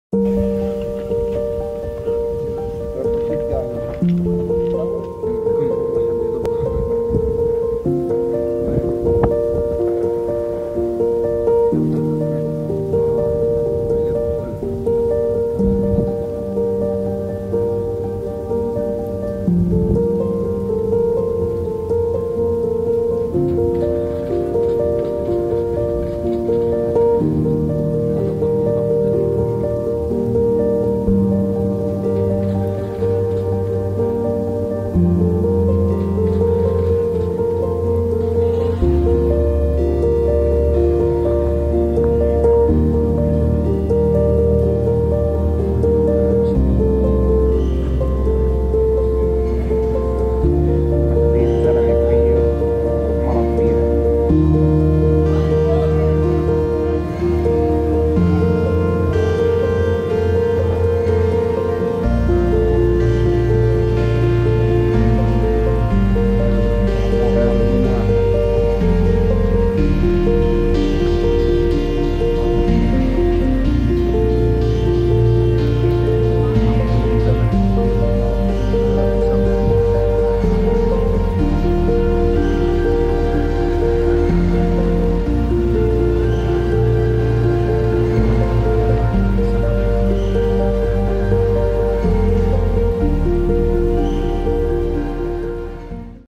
"اعتقالات وغاز مسيل للدموع".. قوات جيش الاحتلال تقتحم مخيم شعفاط بالقدس
قوات_جيش_الاحتلال#
الاحتلال_الإسرائيلي#